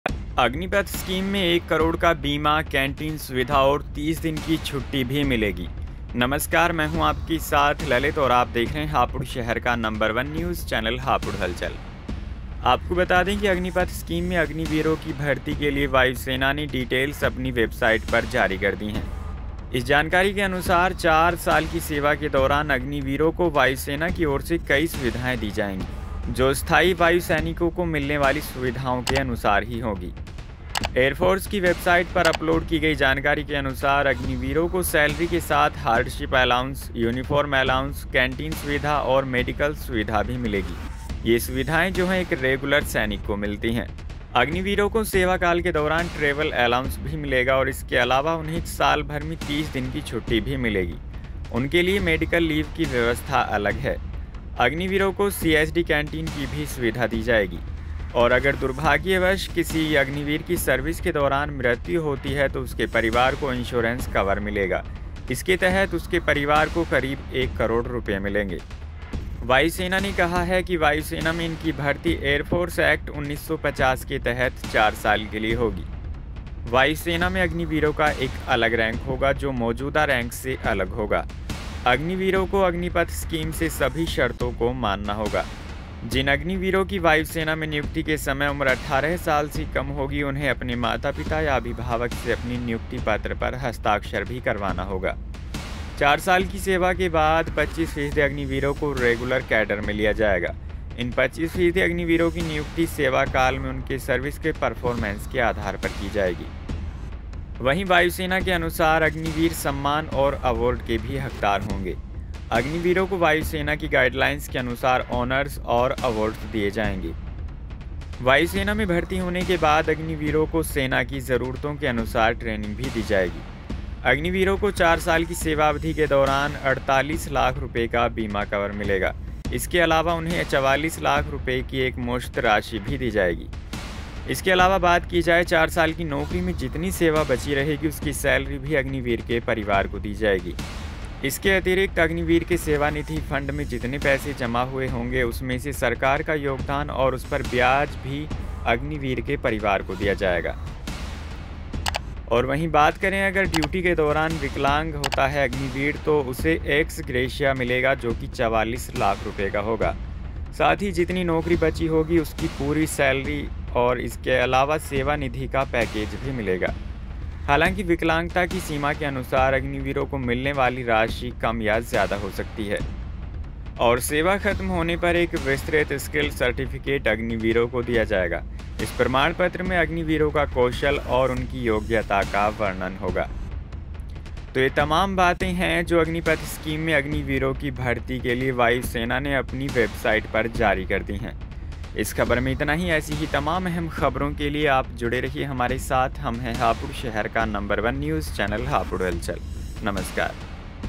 Agnipath Scheme Updtae : अग्निपथ स्कीम में 1 Crore का Insaurance -कैंटीन सुविधा-30 दिन छुट्टी
अग्निपथ स्कीम में अग्निवीरों की भर्ती के लिए वायुसेना ने डिटेल अपनी वेबसाइट पर जारी कर दी है. इस डिटेल के अनुसार चार साल की सेवा के दौरान अग्निवीरों की वायुसेना की ओर से कई सुविधाएं दी जाएंगी जो स्थायी वायुसैनिकों को मिलने वाली सुविधाओं के अनुसार ही होगी.

Team :- HHN
Edit By - NAITIK SAINI
Anchor :- lalit